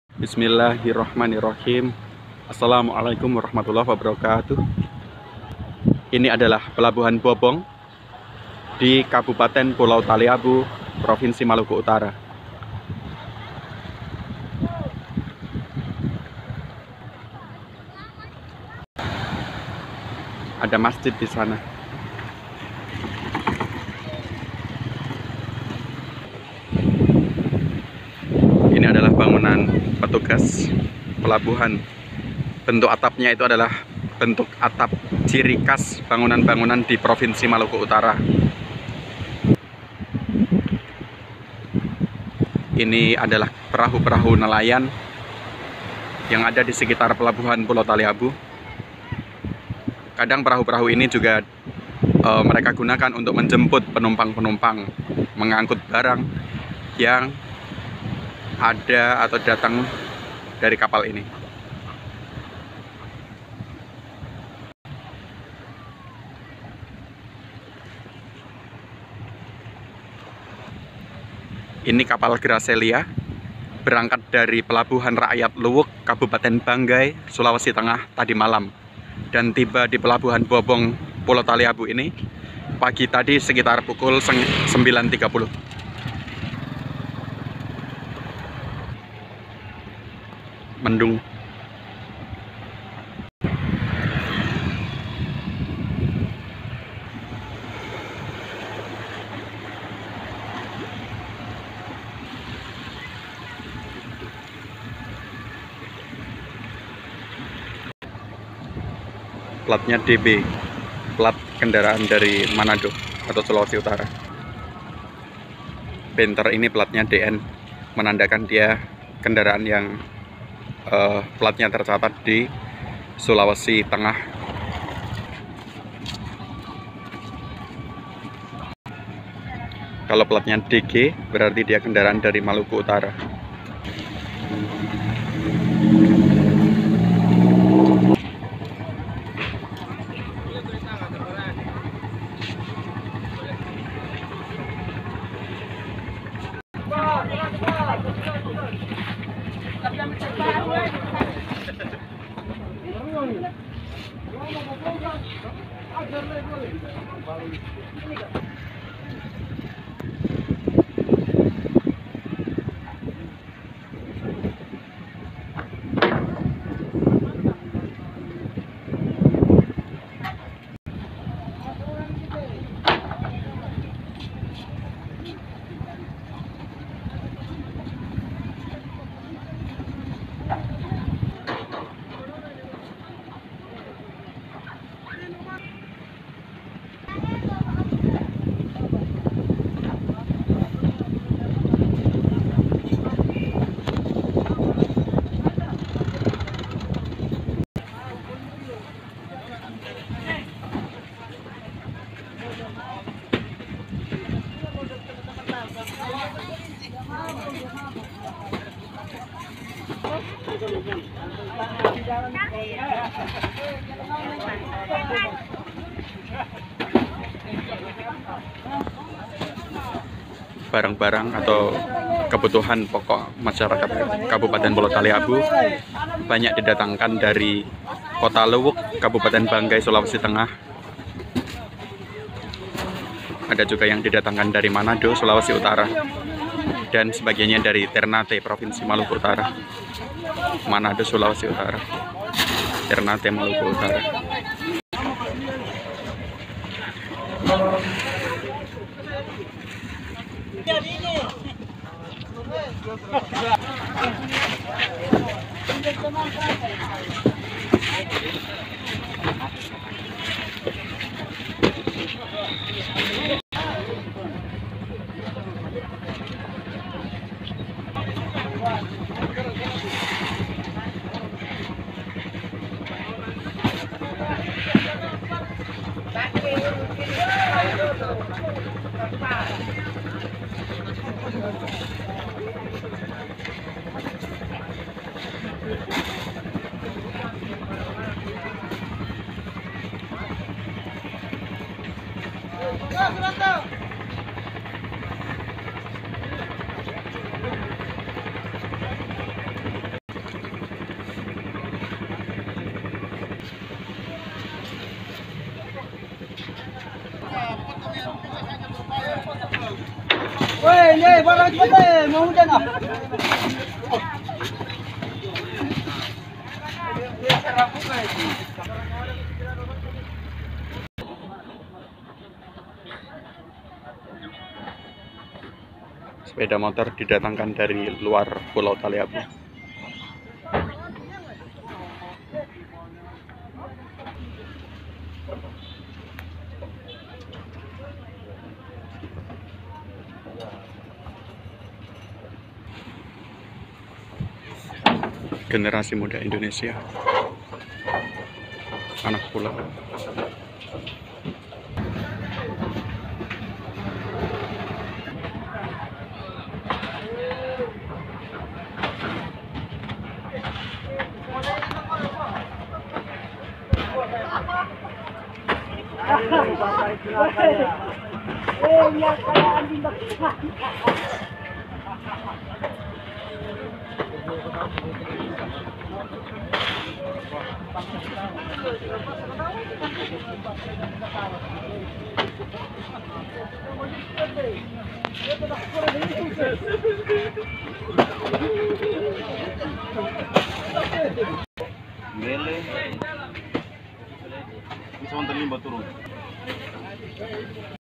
Pelabuhan Bobong, Kab. Pulau Taliabu, Provinsi Maluku Utara
Suasana Pelabuhan Bobong, pelabuhan di ibukota Kabupaten Pulau Taliabu, Maluku Utara, saat kedatangan Kapal Gracelia dari Luwuk, Kab. banggai, Sulawesi Tengah.